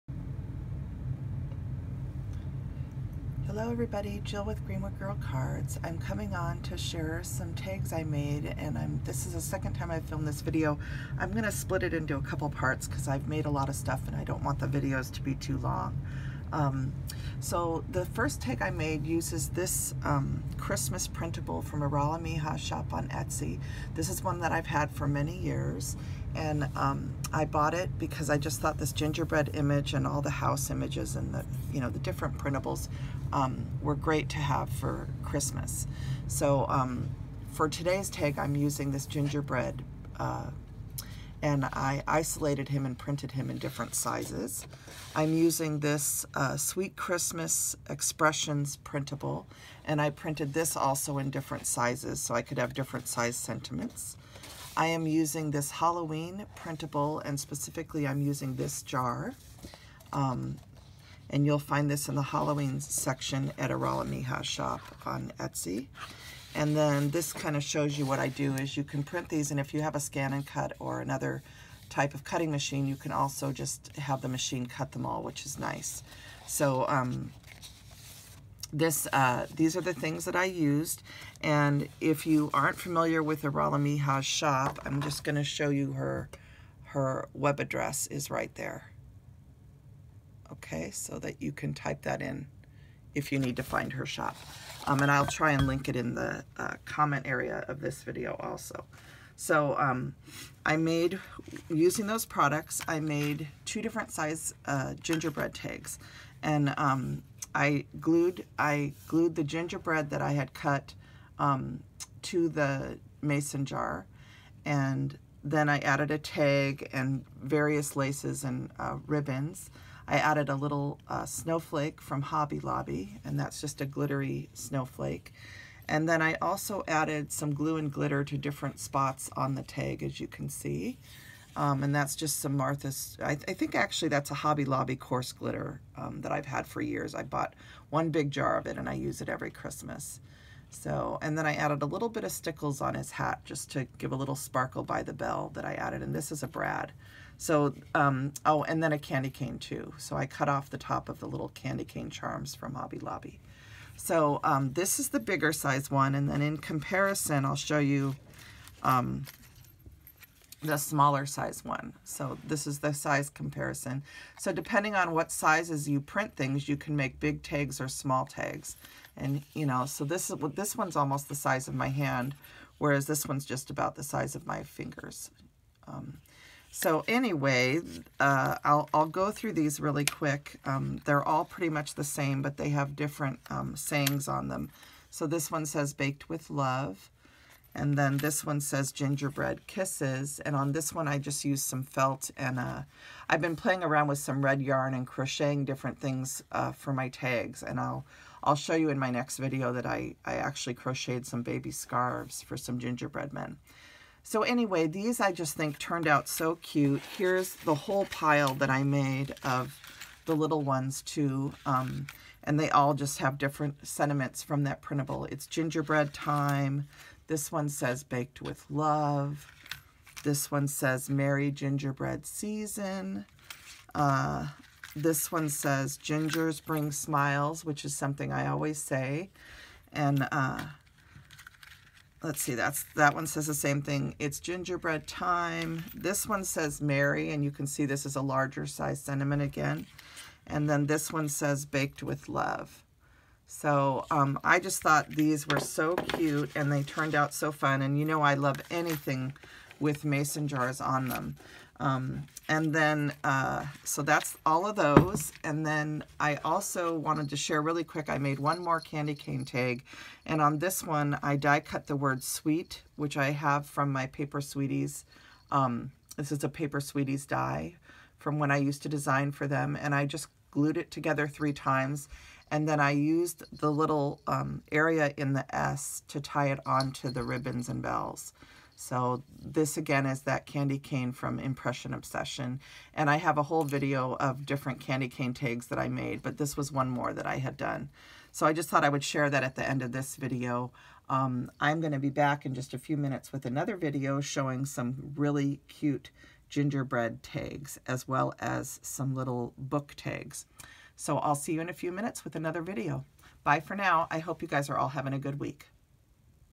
Christmas Tags 2020: Gingerbread Men and Mason Jar Tags
Christmas Tags 2020 continued:  This set of Gingerbread Men and Mason Jar tags is made using various digital printables from Iralamija Shop on Etsy.
Crafty Hugs,
Jill

Iralamija Shop at Etsy dot com: